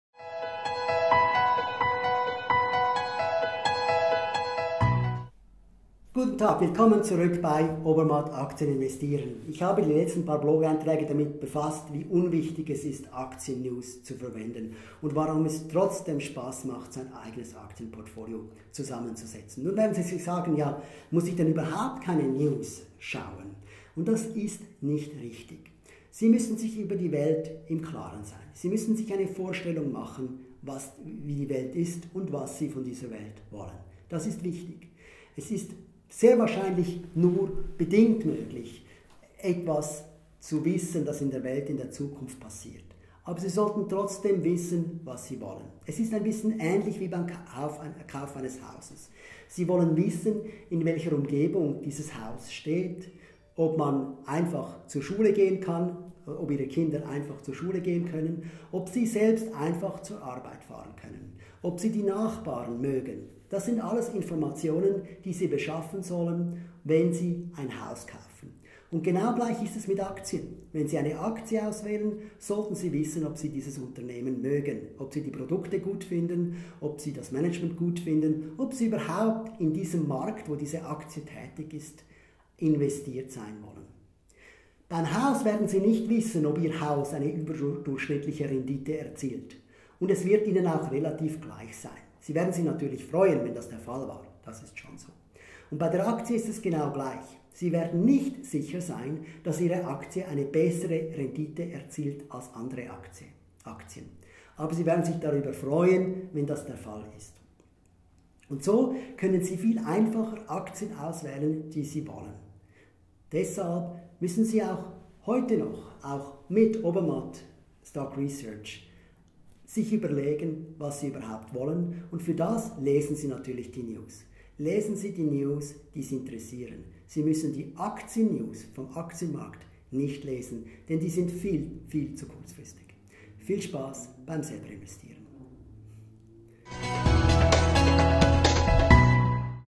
Der Aktienkauf ist wie der Hauskauf
Man kann den Aktienmarkt kaum schlagen - ausser es ist Ihre Berufung. Meine ist es sicher nicht. Deshalb investiere ich, ohne zu versuchen, den Markt zu schlagen. Ich bin zufrieden mit der durchschnittlichen Aktienmarkt-Rendite. Sie ist viel höher als die Rendite auf Anleihen (Obligationen), die Preissteigerung beim Gold oder die Zinsen auf dem Bankkonto. Und zu guter Letzt, mit jedem Aktienkauf werde ich Teilbesitzer eines Unternehmens, das ich mag, und das gefällt mir.

Deshalb investiere ich in Unternehmen, die ich gut finde, in geografische Märkte, die mich interessieren und in Unternehmen, die gute Produkte verkaufen und ähnliche Werte wie ich verkörpern.
Für mich ist der Aktienkauf ähnlich wie der Hauskauf: Ich entscheide mich für mein Haus, nachdem ich mich über die Nachbarschaft, die Geografie und das Haus ins Klare versetzt habe. Ich schaue also, ob alles für mich passt, bevor ich das Haus kaufe.

Wenn ich ein Haus zum Wohnen kaufe, dann sind zukünftige Preiserhöhungen nicht meine Motivation, denn das kann man auch kaum vorhersagen. Ich kaufe ein Haus, weil es meinen Zwecken dient. So sollte es auch mit Aktien sein. Ich kaufe Aktien, weil sie mit meinem Geld arbeiten. Wenn sie die Leistung anderer Aktien übertreffen, ist das mein gutes Glück, aber nicht mein Motiv. Mein Motiv ist eine faire Rendite.

Wenn man sich die Aktieninvestition wie der Hauskauf vorstellt, dann ist der Aktienkauf viel einfacher.